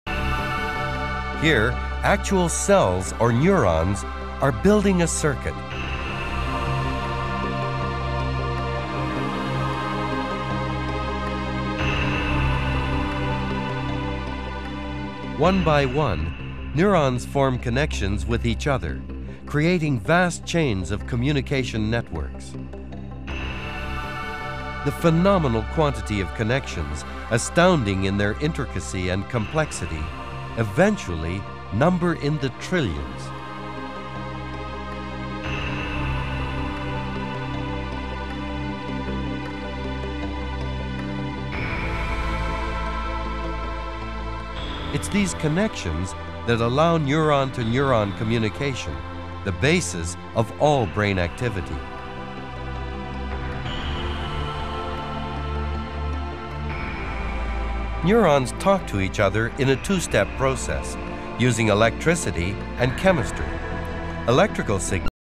神經細胞連結是大腦學習及記憶的基礎
神經連結語學習能力息息相關